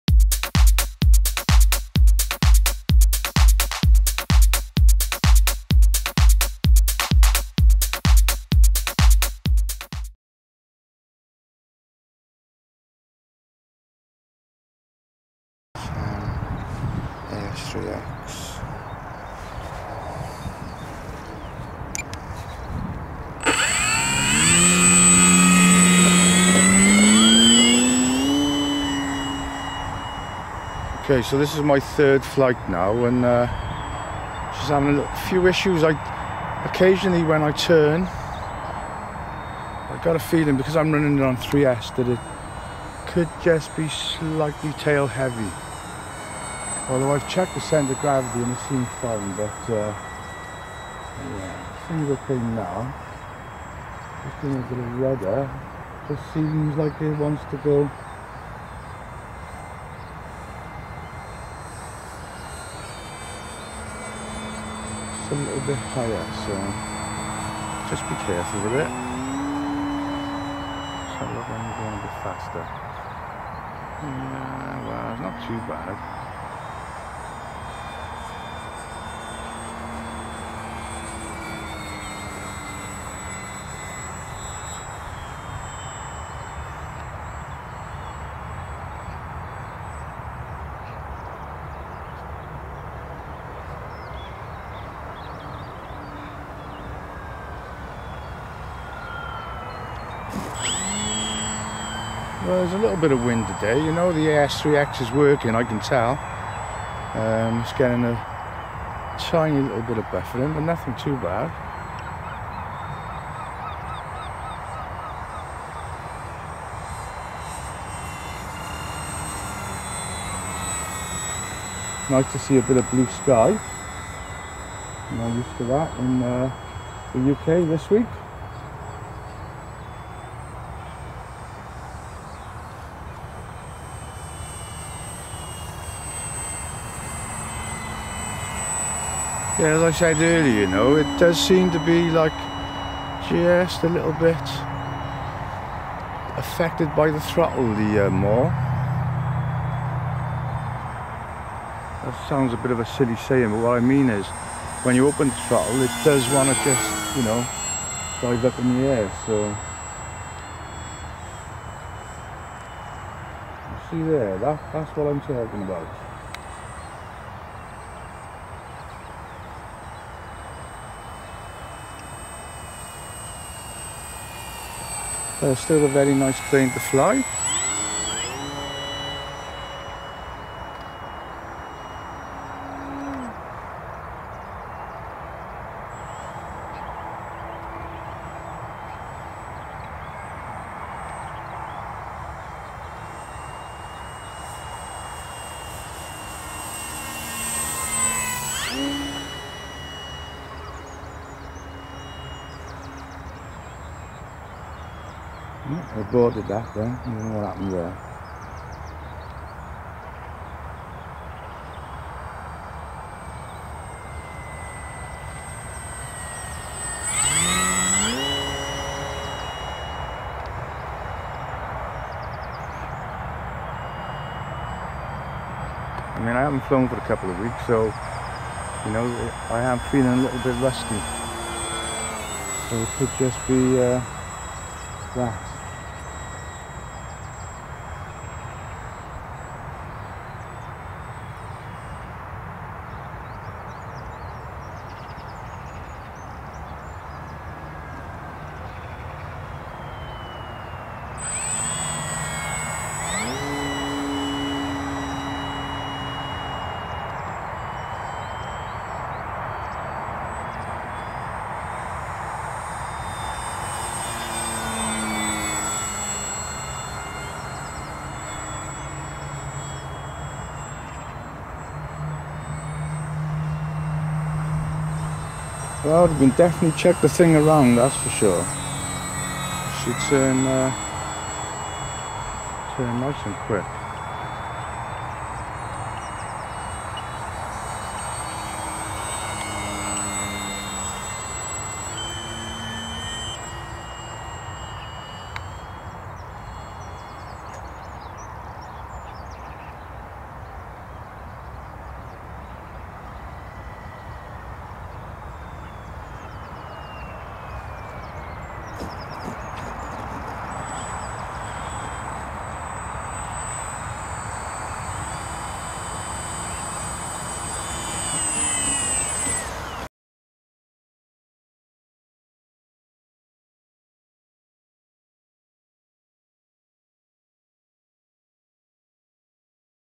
E flite M7 MAULE FLIGHT 3
Third flight from this morning with the Maule.